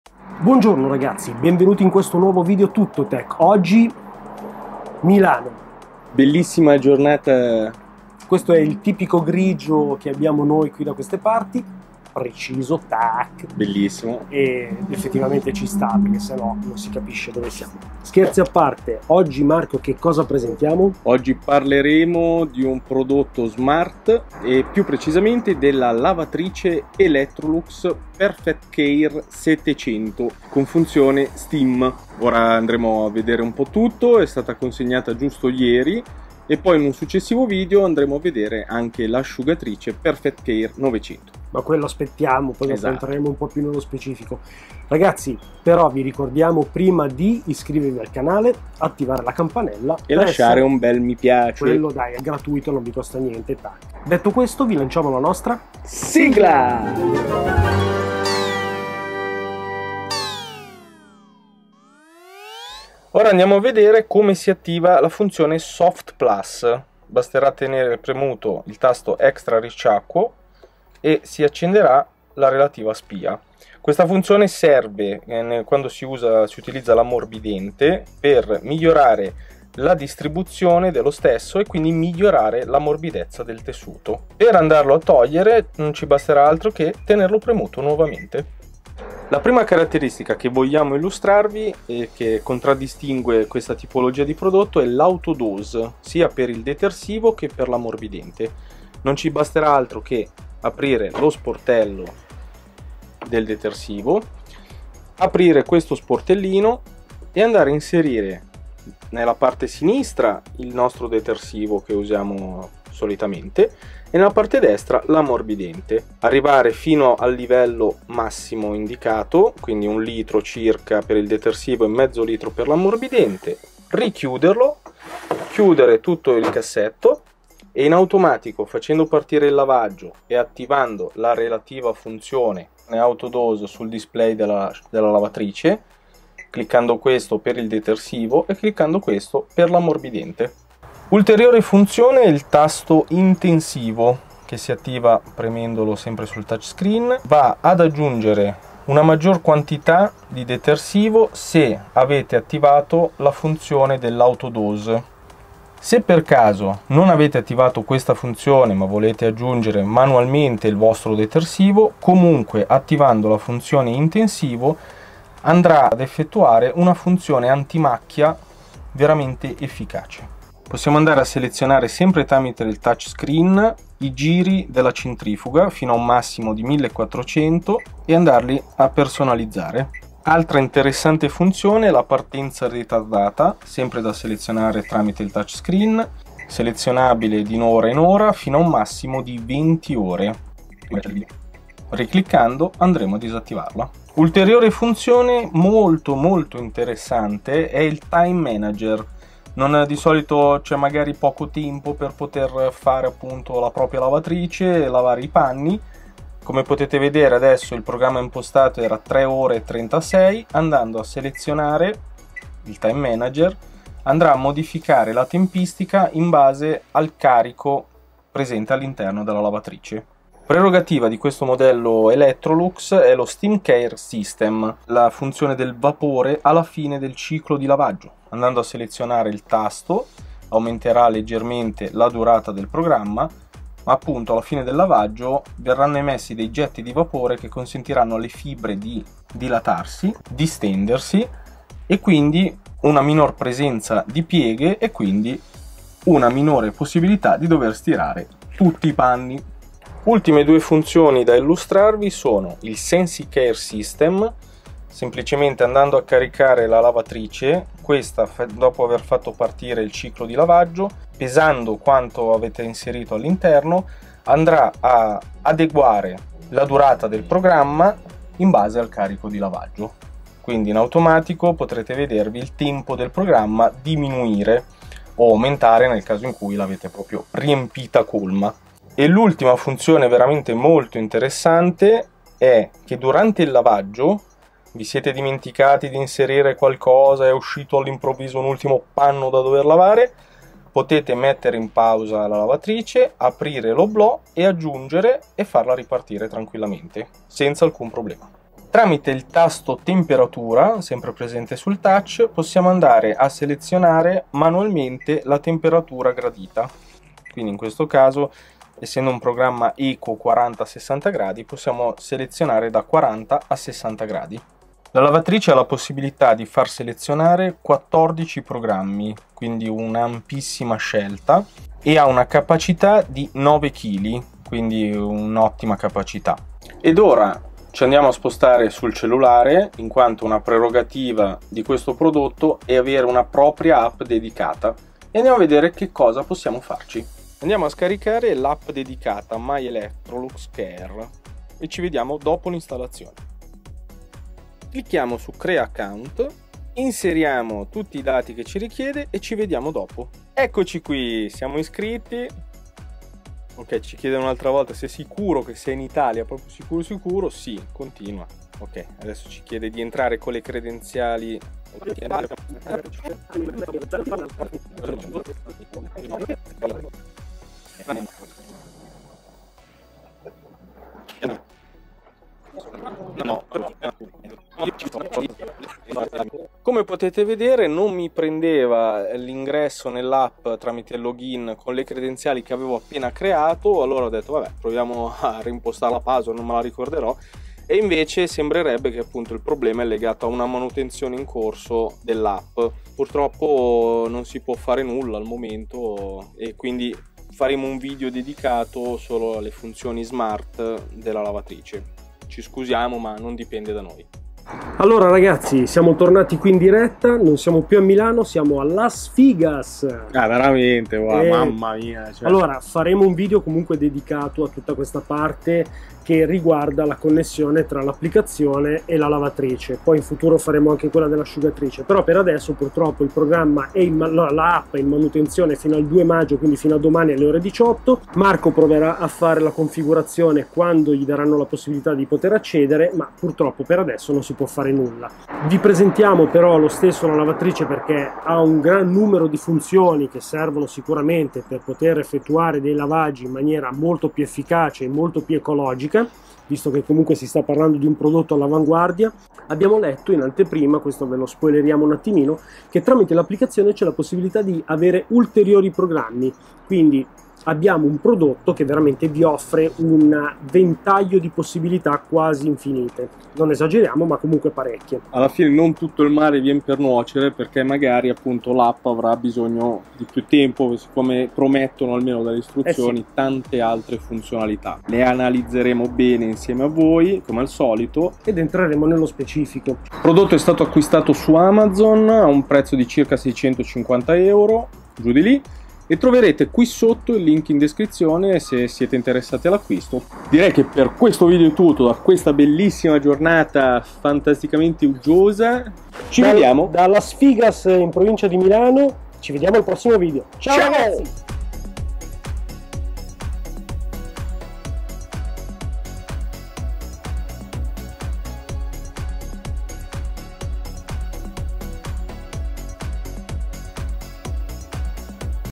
LAVATRICE SMART ELECTROLUX, FA TUTTO LEI O QUASI! ITA
✰ LAVATRICE ELECTROLUX